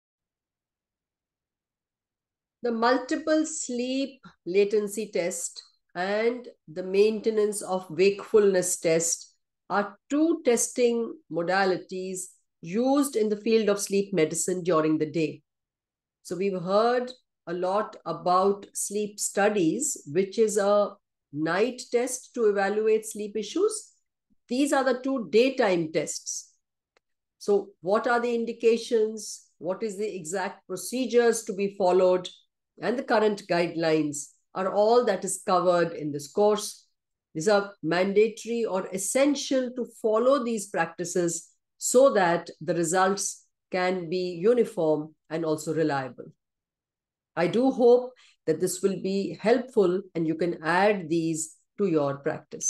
Upskill in Multiple Sleep Latency & Maintenance of Wakefulness Test with Dr.Manvir Bhatia
Don't miss this wonderful opportunity to learn from subject matter expert, Dr.Manvir Bhatia.
Upskill and reskill yourself in the Multiple Sleep Latency Test & Maintenance of Wakefulness Test. Dr.Manvir is a Senior Neurologist and Sleep Specialist with more than 30 years of experience & currently the Director of Neurology & Sleep Centre, New Delhi. She is also the Vice President of the Indian society of Sleep Research & has authored the book-“The Sleep Solution” the first non-fiction book on sleep, in India.

Key Program Highlights
·        Get more confident in how to select your patient for MSLT-MWT testing
·        Attain an e-certificate on module completion
·        Learn at your own pace from meticulously curated content.

Who Should Enroll?
For medical professionals who want to advance their careers with a deeper level of understanding of sleep medicine.

Enroll on or before 5th July& avail of the limited period discount offer!!

To know more about the program, have a look at the flyer and video-